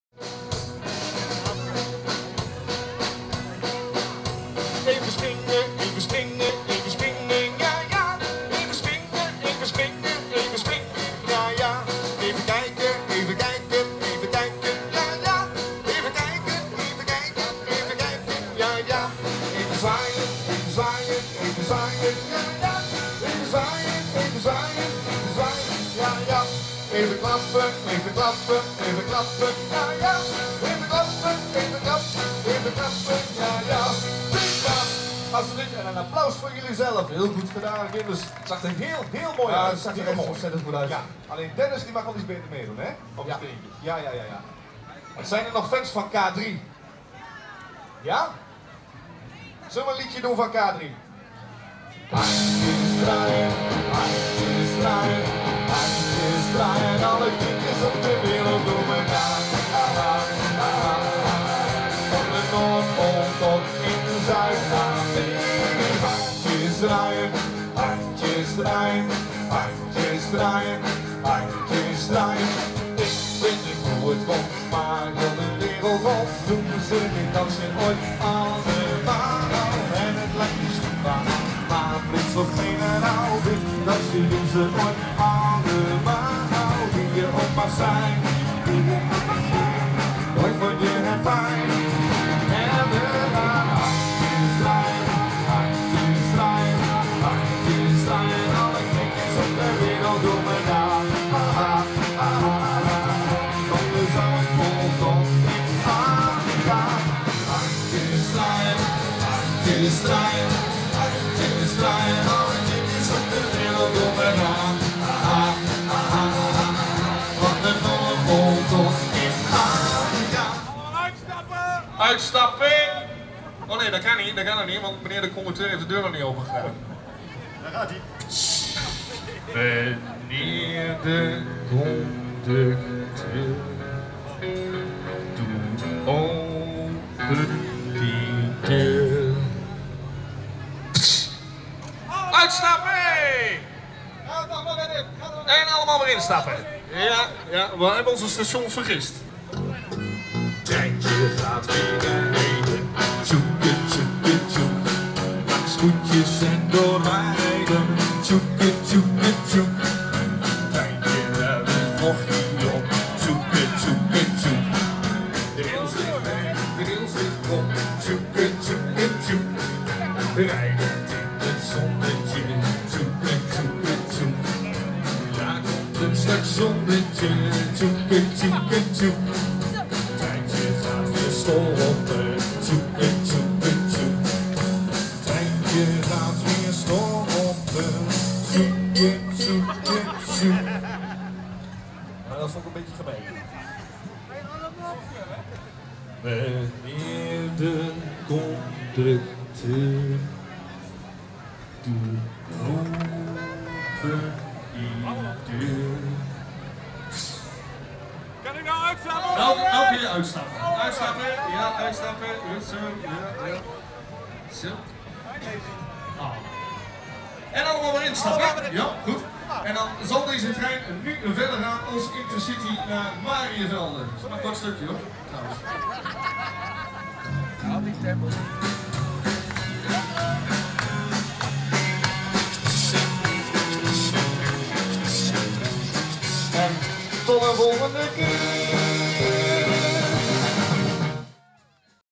Plork en de Aannemers Blagenparadijs
Plork en de Aannemers op het Blagenparadijs op de Zwarte Cross 2013, gefilmd door Focuz TV